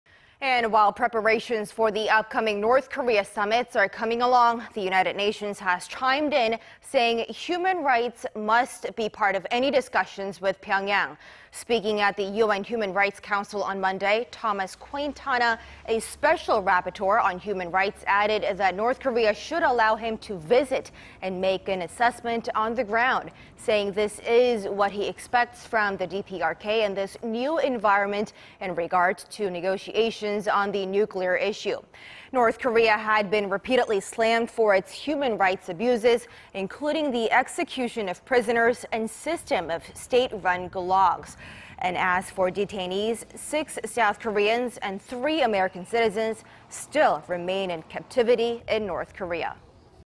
Human rights must be part of any talks with North Korea: UN Investigator
유엔보고관, "북한 인권, 핵 문제와 함께 논의돼야"
  And while preparations for the upcoming North Korea summits are coming along,... the United Nations has chimed in,... saying human rights *must be part of any discussions with Pyongyang.
  Speaking at the UN Human Rights Council on Monday,... Thomas Quintana, a special rapporteur on human rights added that North Korea should allow him to visit and make an "assessment on the ground," saying this is what he "expects from the DPRK in this new environment in regard to negotiations on the nuclear issue."
North Korea had been repeatedly slammed for its human rights abuses,... including the execution of prisoners and system of state-run gulags.
And as for detainees,... six South Koreans and three American citizens remain in captivity in North Korea.